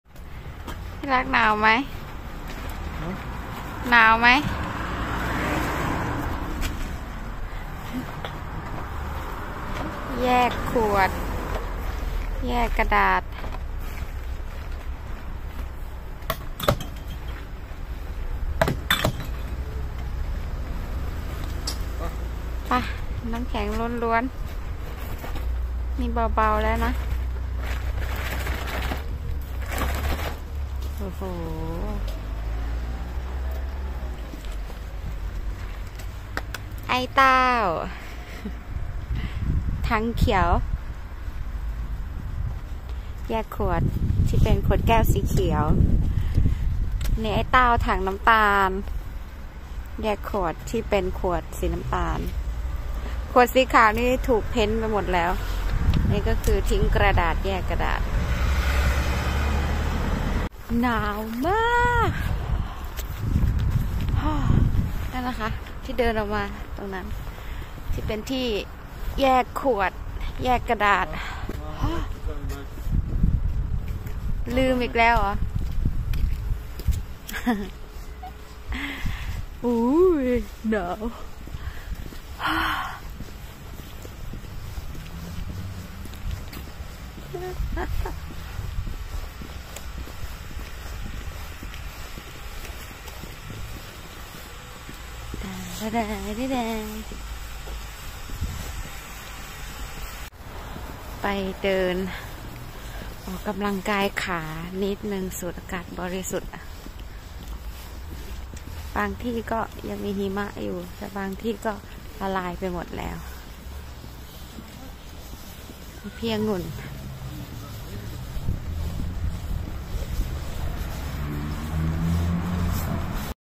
เมื่อหิมะหยุดตก ก็พากันมาแยกขยะ
หนาวๆ อากาศ -3 องศา ❄️ หิมะตกเพิ่งหยุดไป
พากันเดินมาทิ้งขวด แยกกระดาษ 😄

กิจกรรมที่ทำเป็นประจำ